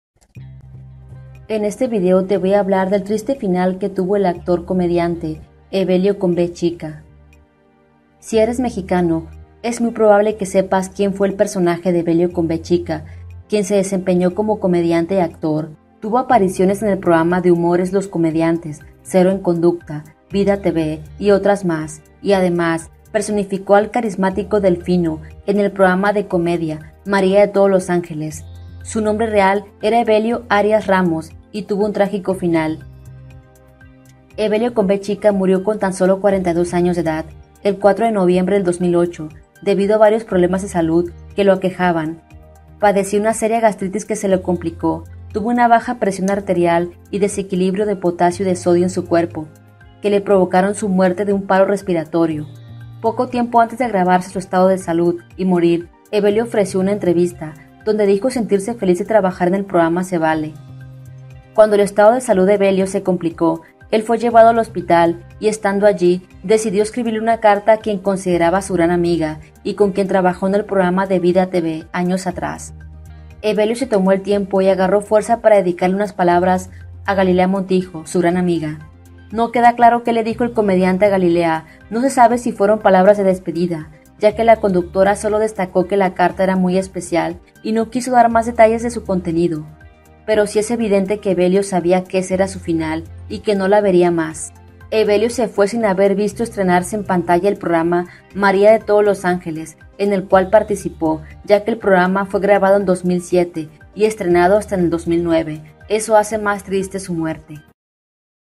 El triste final del actor y comediante Evelio con V chica | Le dejó una carta a Galilea.
En este vídeo te hablaré del triste final que tuvo Evelio Con v chica, a sus apenas 42 años de edad. Sufrió una repentina enfermedad. Antes de morir le dejó una carta a quien fue su gran amiga, Galilea Montijo. Evelio perticipó en varios programas populares de la televisión mexicana; en Vida Tv, Humor es los comediantes, María de todos los ángeles, etc. Sin duda, se le extraña. Era muy simpático.